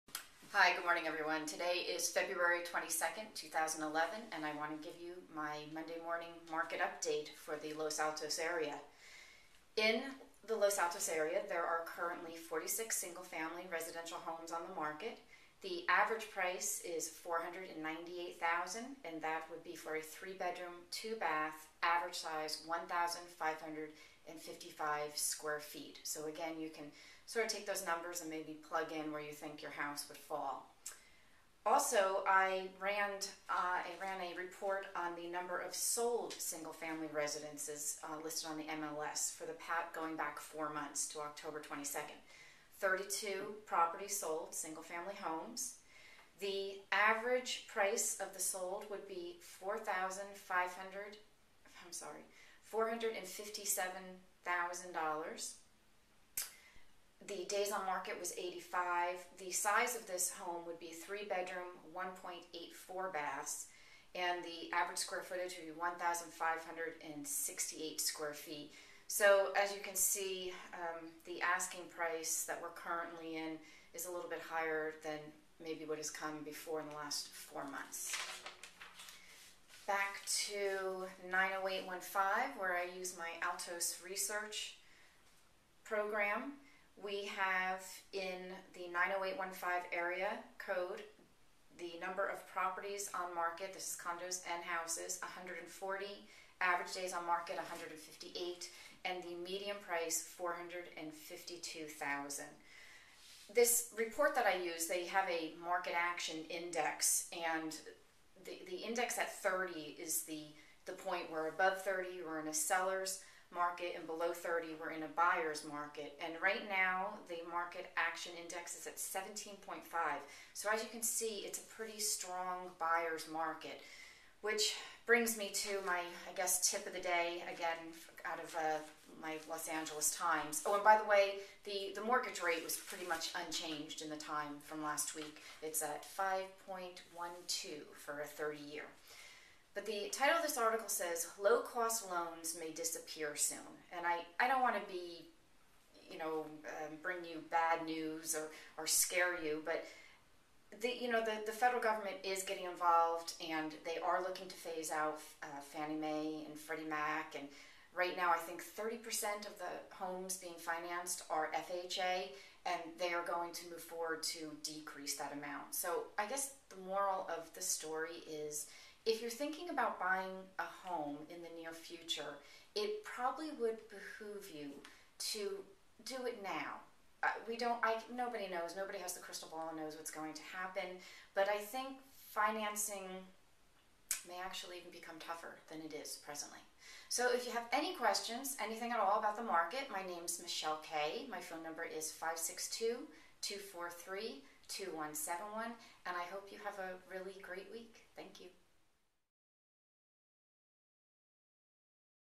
Los Altos Market Update Feb 22, 2011
According to the MLS, the Los Altos area currently has 46 homes on the market with an average asking price of $497,876  and an average size of 3 bedroom/2 bath and 1,555 sq ft.

There have been 32 single family residences sold during the last 4 months in the Los Altos area with an average sold price of $456,992 with an average size of 3 bedrooms/l.84 baths and 1,568 sq ft of living space. Average Day on Market (DOM) was 85

Altos Research for the 90815 zip code shows a total of 140 properties on the marketing with a median list price of $452,500 and average DOM is 158.

The Market Action Index is currently at 17.51 which is far below 30 which is the break point between a sellers and a buyers market. Clearly we are in a buyer's market.

Los Angeles Times published mortgage rate is 5.12% for a 30-year fixed, which is virtual unchanged from the prior week.

My tip for the week comes from an LA Times article by Kenneth Harney titled, "Low-cost loans may soon disappear". Currently you can get a 30 year fixed mortgage in the 5% range with minimum down payments below 5%. Jumbo loans still can be obtained at regular interest rates. Harney poises that this scenario will soon change and we will be required to pay more for mortgages in the future.  If you are thinking about buying in the near future but have put off the exercise for a variety of reasons, now my be the time to stop thinking about buying start the process of buying.

I Know Long Beach and I Love Los Altos!